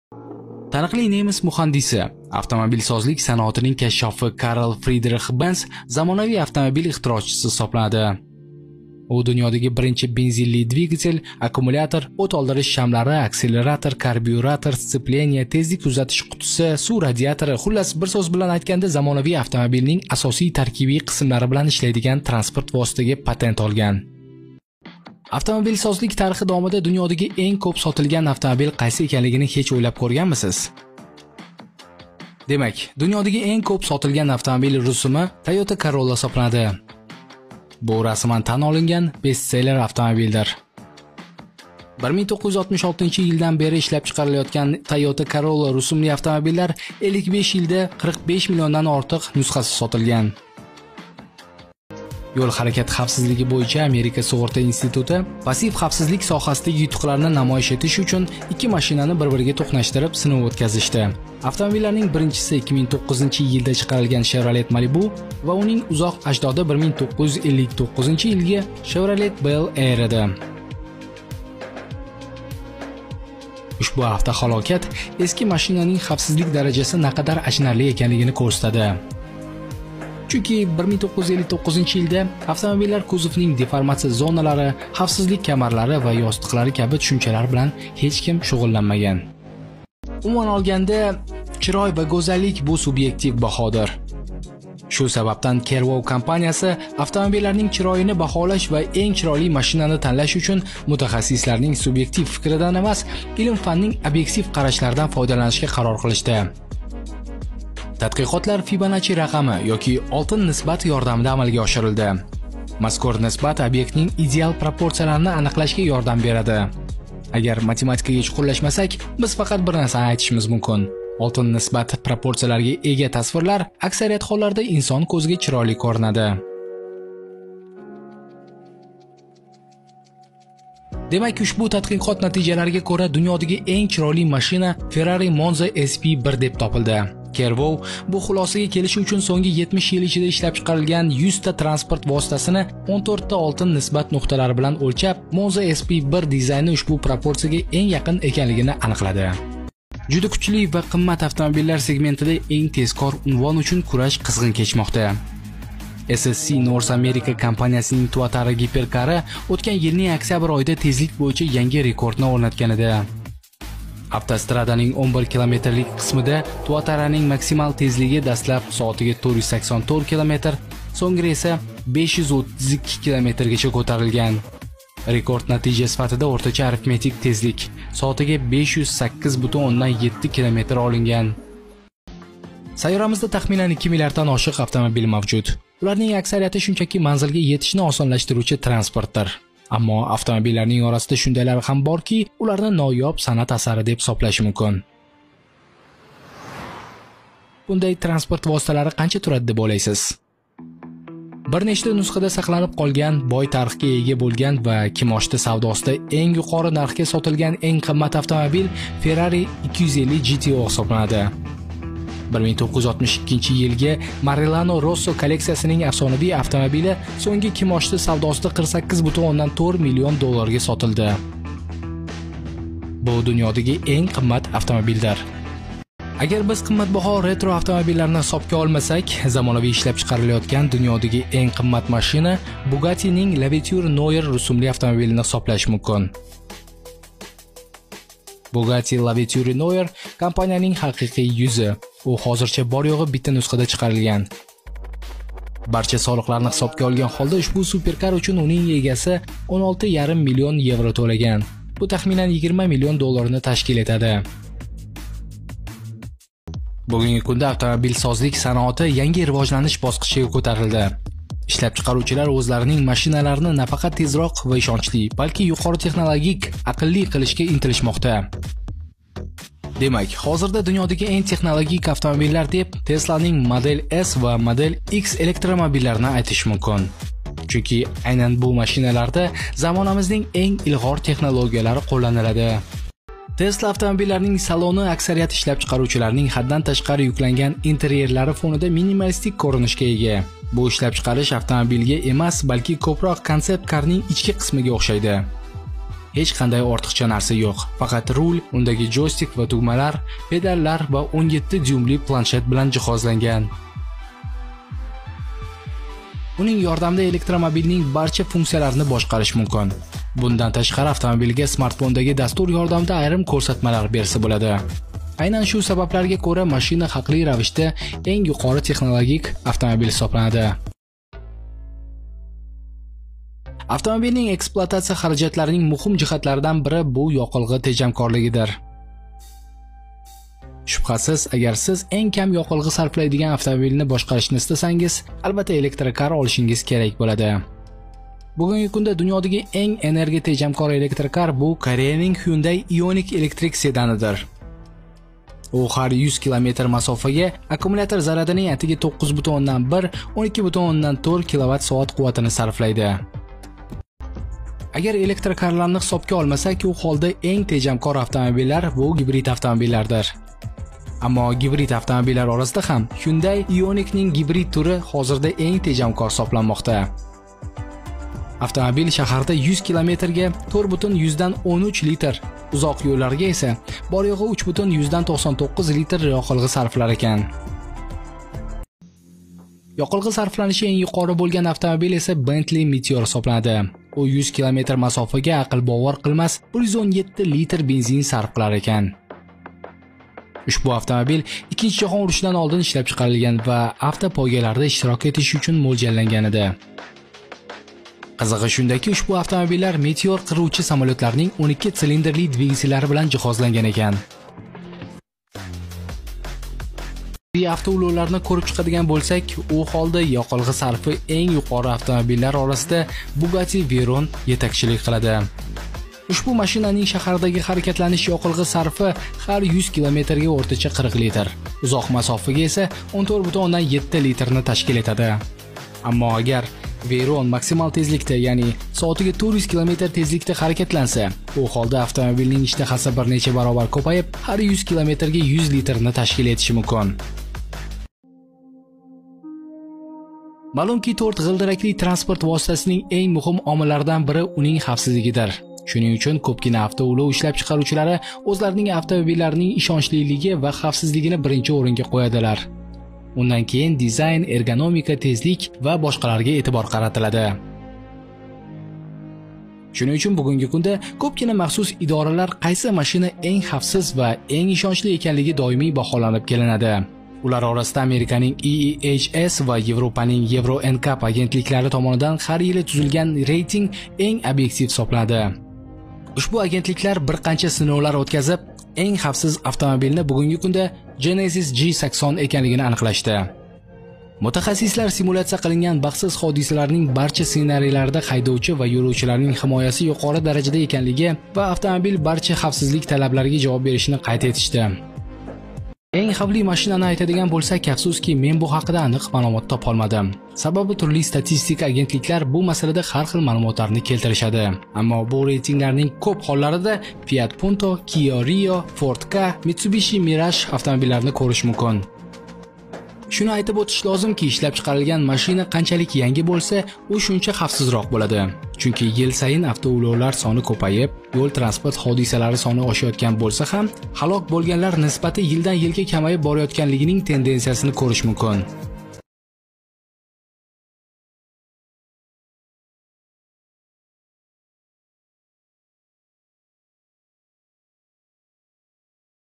Dunyodagi eng arzon, eng qimmat, eng tez, eng yaxshi va eng tezkor avtomobillar xaqida maʼlumot.
Eng birinchi avtomobildan eng soʻnggi mashinalargacha boʻlgan maʼlumotni sizga taqdim etamiz. kanalga obuna boʻling va siz ham Flegmatik TV oilasiga qoʻshiling:)